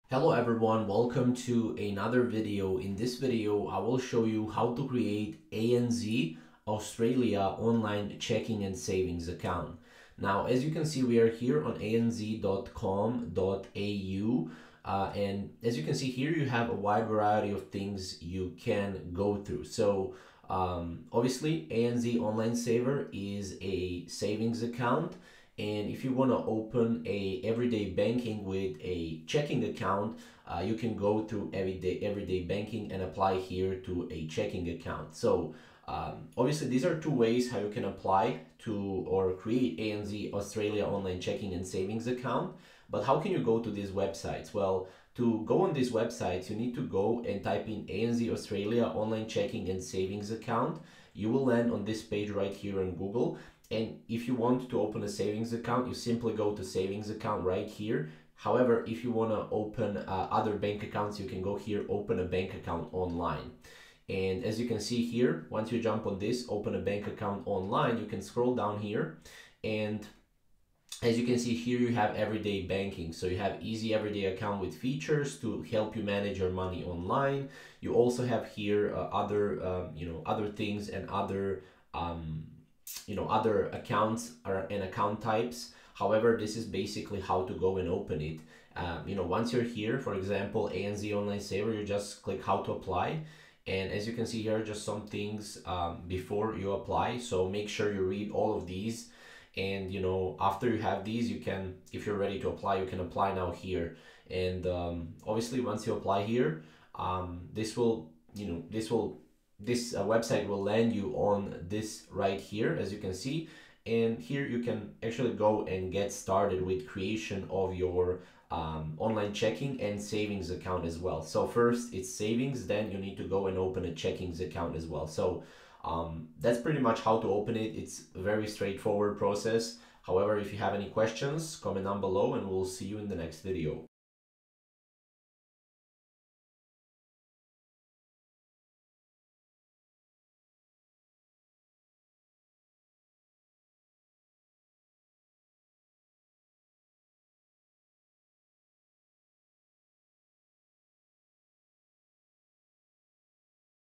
How To Create ANZ Australia Online Checking And Saving Account (Tutorial)
Learn how to Create ANZ Australia Online Checking And Saving Account